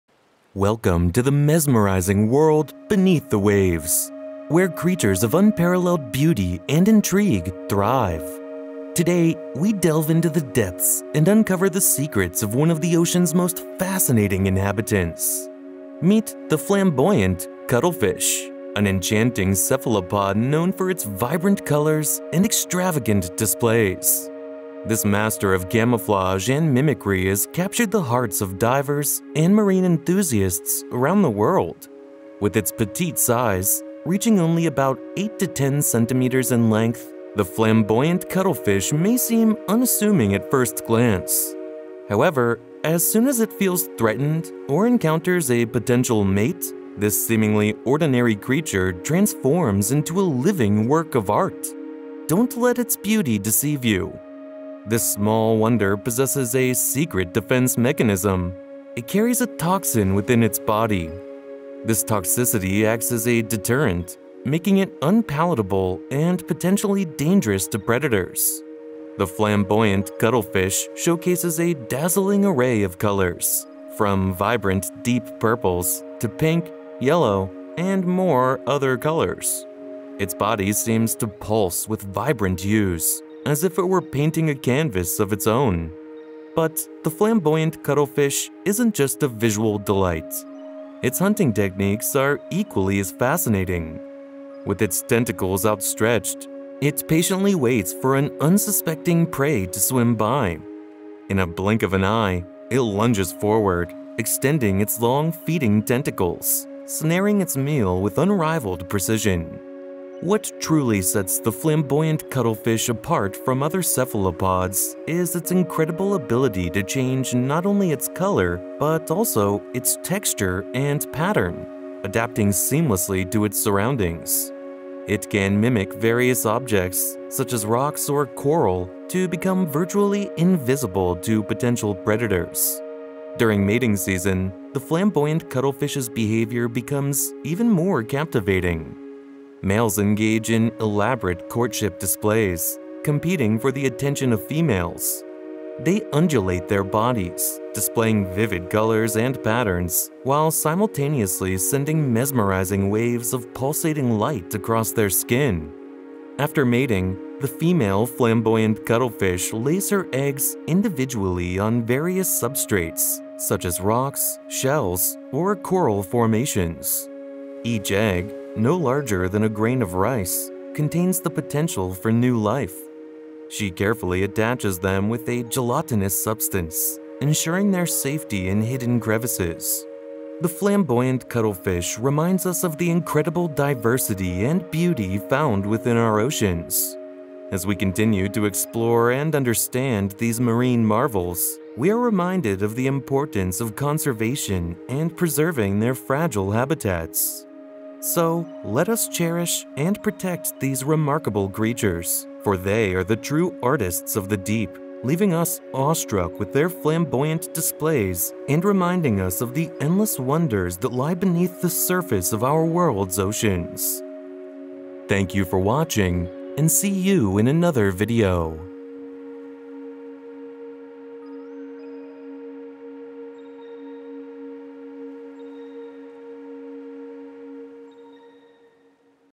Marine Wonders: Flamboyant Cuttlefish
The flamboyant cuttlefish (Metasepia pfefferi) is a captivating cephalopod known for its vibrant color displays and stored toxin in its flesh. More fascinating details can be found in the video. Enjoy the mesmerizing beauty of this living work of art beneath the waves!